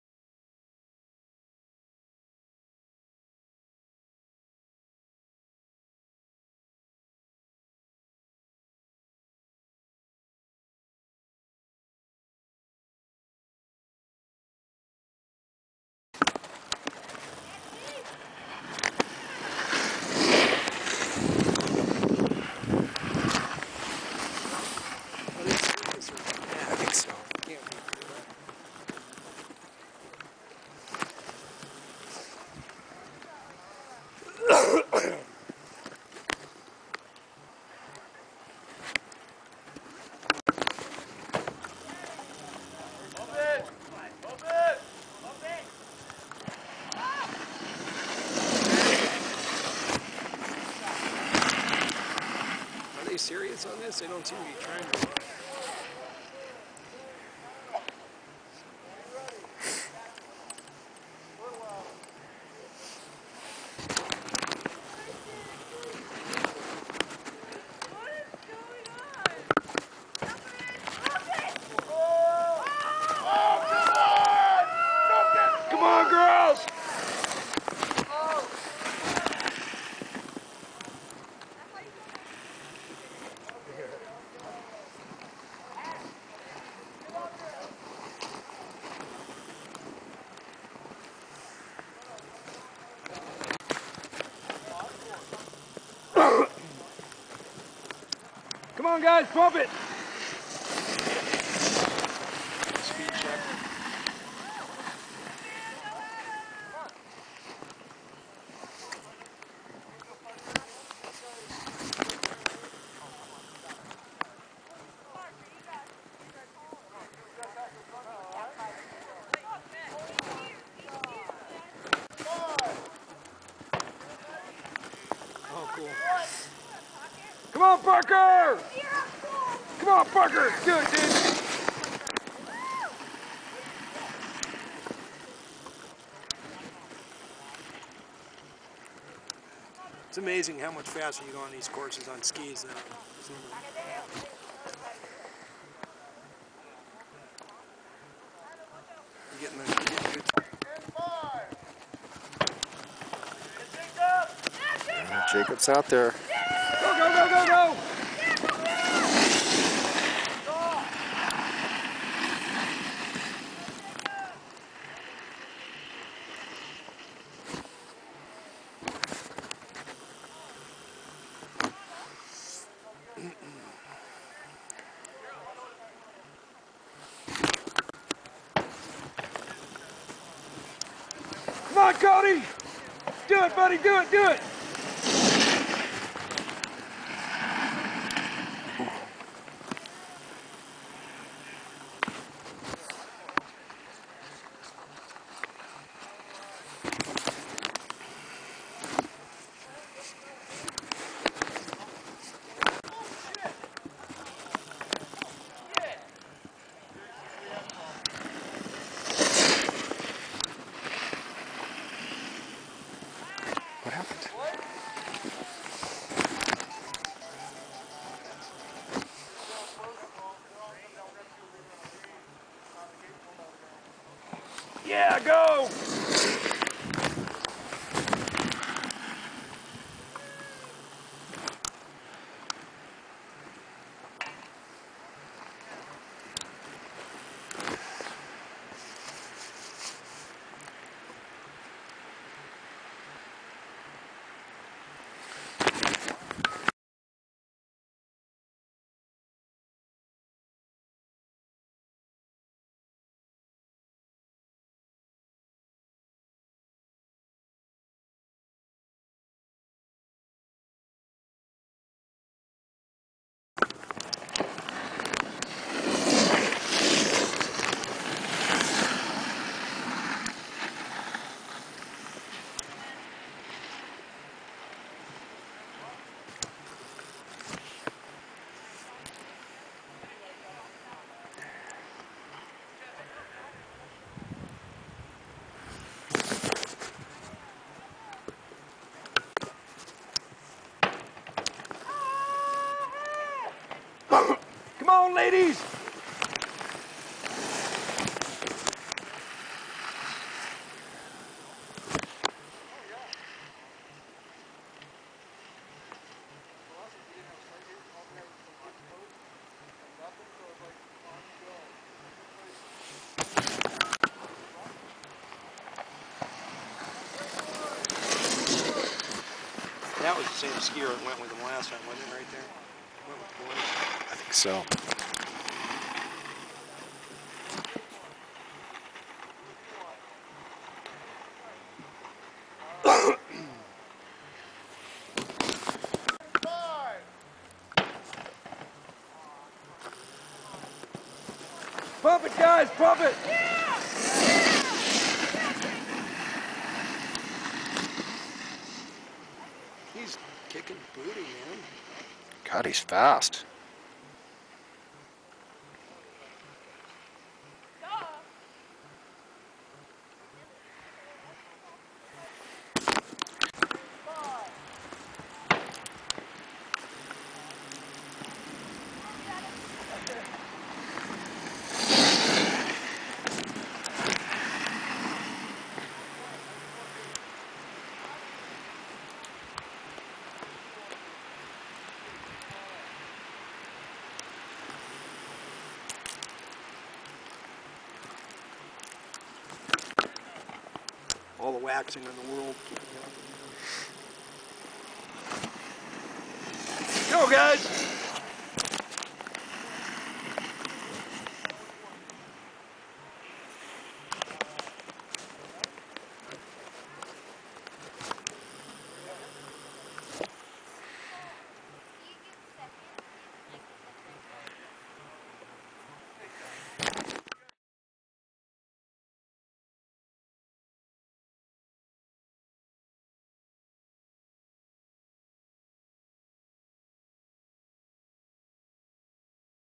USASA Hood series BX 02/28/2009 part 1
full runs of the three boardercross events.
USASA Hood series boardercross
Timberline, Mt. Hood, Oregon